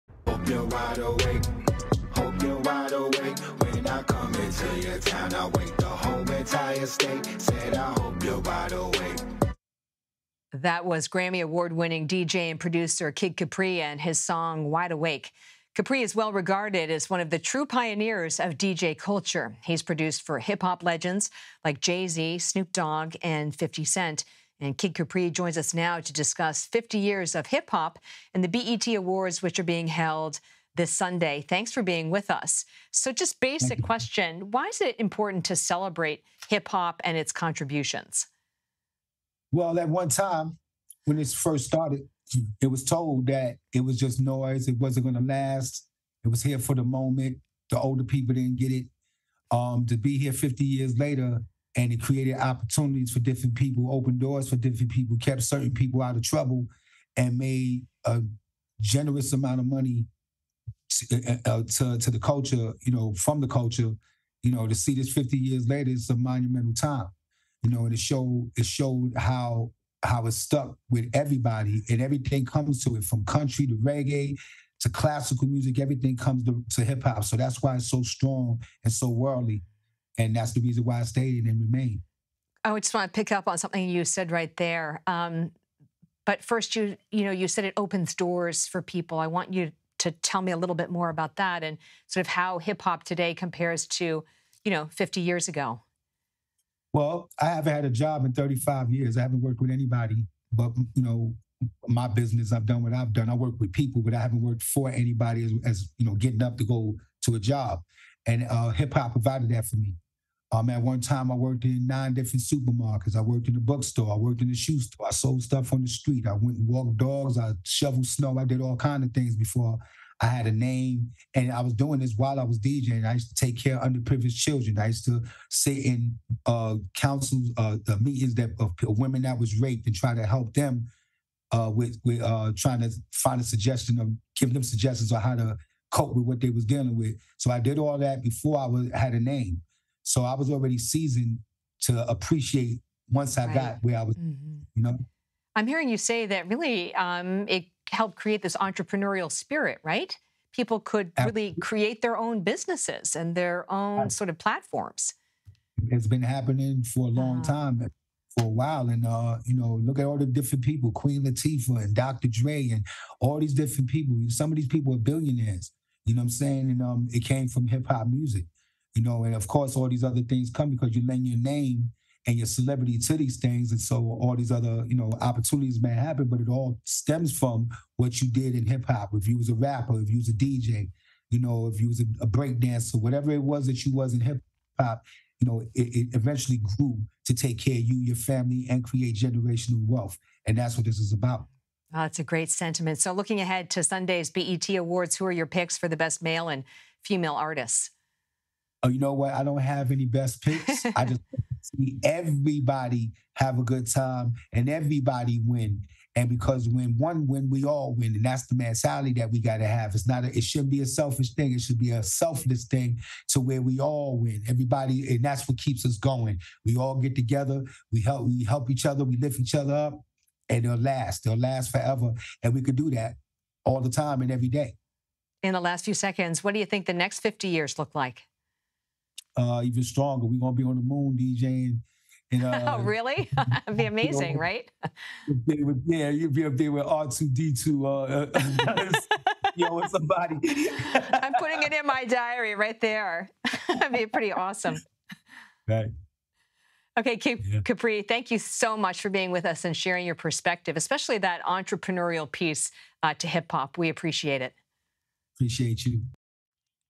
Kid Capri reflects on 50 years of hip-hop ahead of BET Awards
2023 marks 50 years since the creation of hip-hop. Grammy Award-winning DJ and producer Kid Capri joins CBS News to discuss the genre's contributions to the music industry, and his picks for best male and female artists at Sunday's BET Awards.